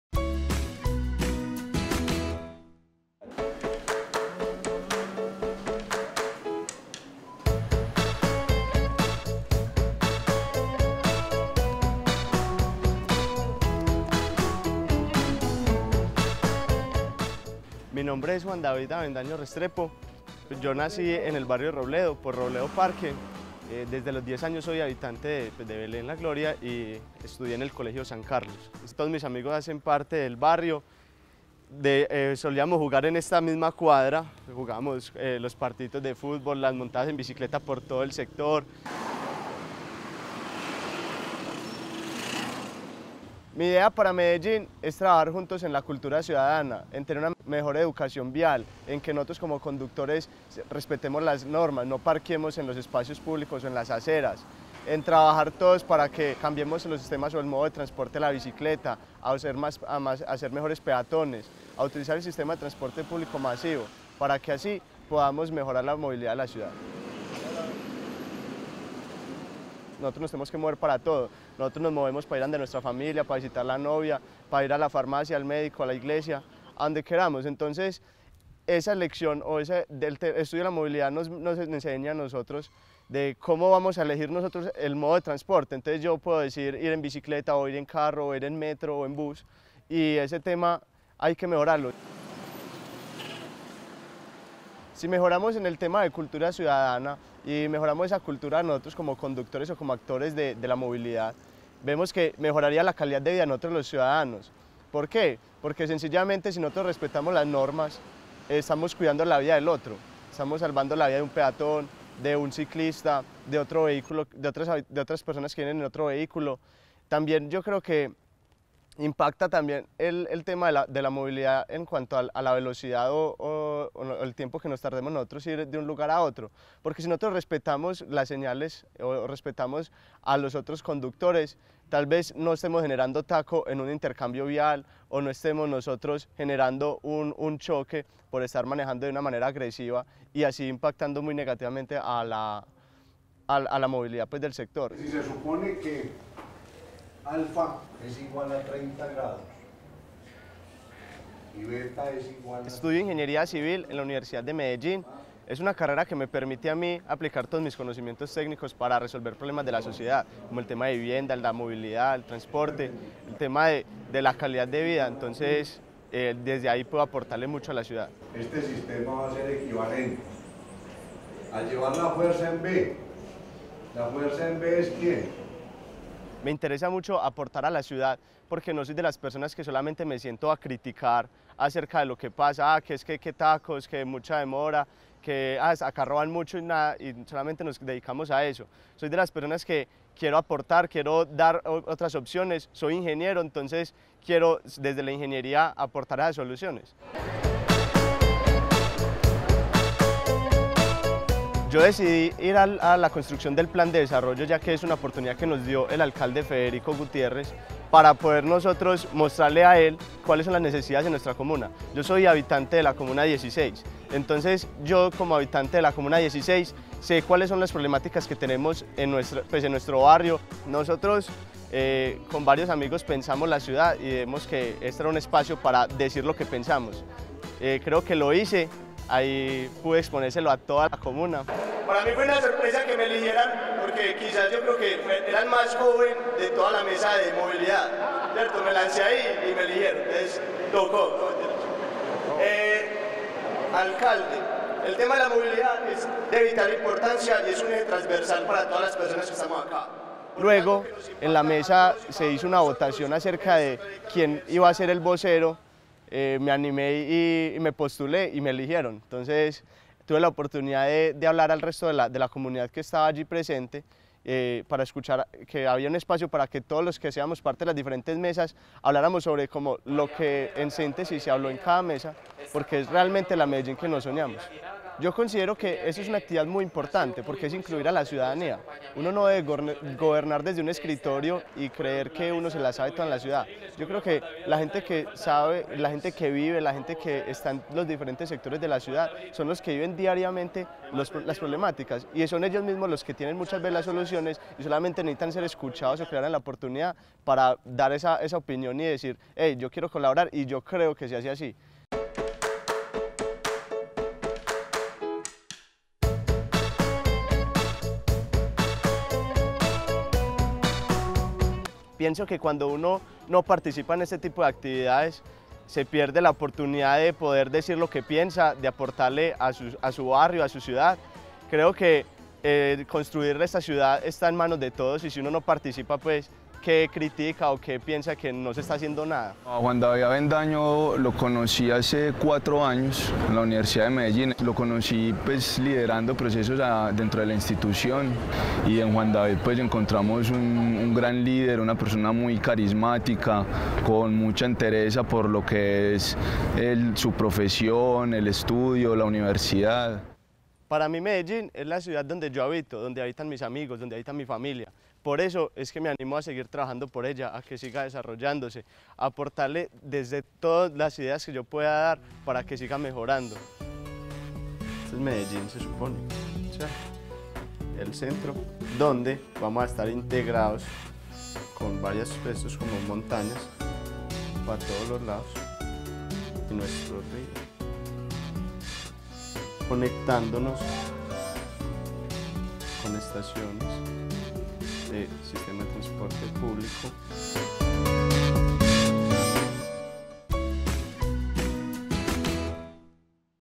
Juan David estudia para transformar
En el barrio Belén La Gloria, de la comuna 16, Juan David Avendaño sueña y trabaja por una mejor ciudad. Este estudiante de Ingeniería civil y líder comunitario tiene muy claro que debemos "trabajar juntos en la cultura ciudadana, tener una mejor educación vial". Aquí te lo presentamos.

Alcalde Federico Gutiérrez Zuluaga